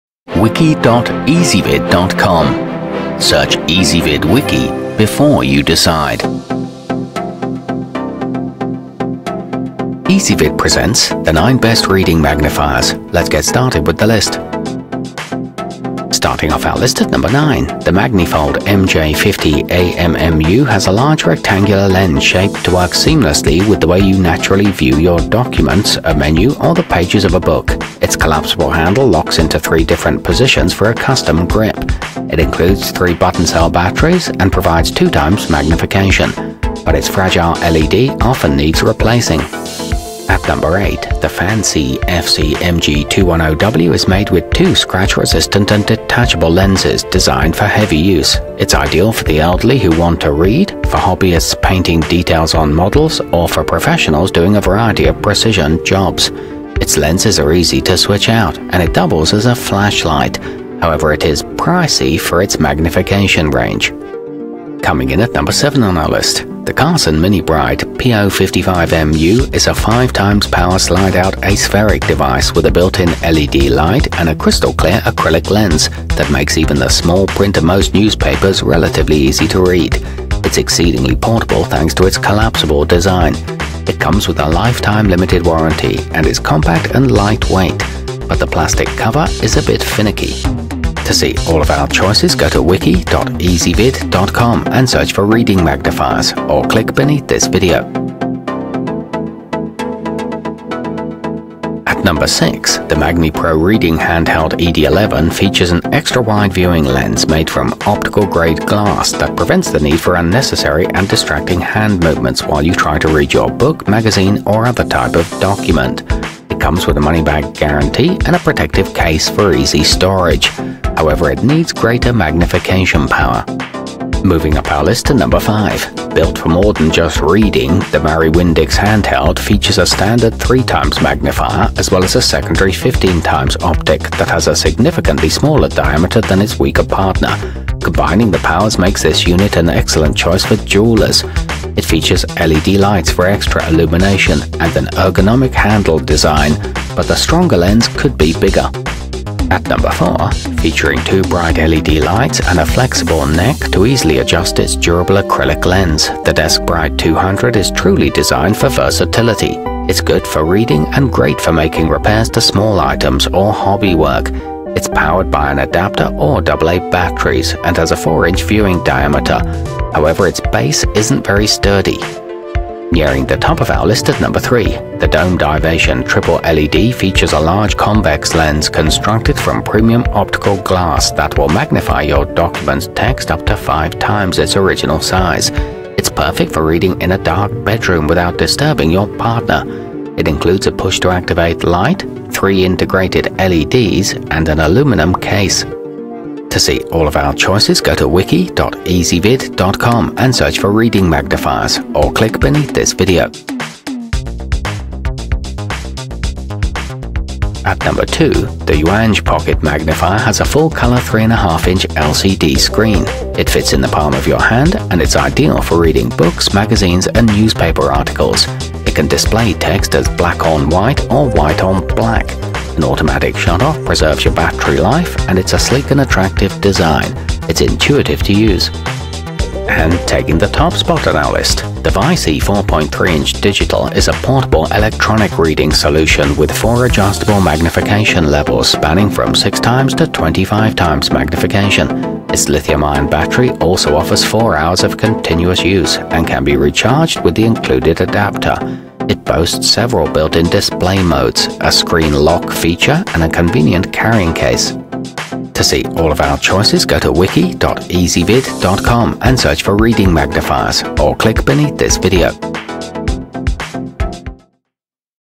9 Best Reading Magnifiers 2017
Reading magnifiers included in this wiki include the fancii fc-mg210w, deskbrite 200, domed ivation triple led, marrywindix handheld, magnipro reading handheld ed11, yuanj pocket magnifier, carson minibrite po-55mu, visee 4.3-inch digital, and magnifold mj-50ammu.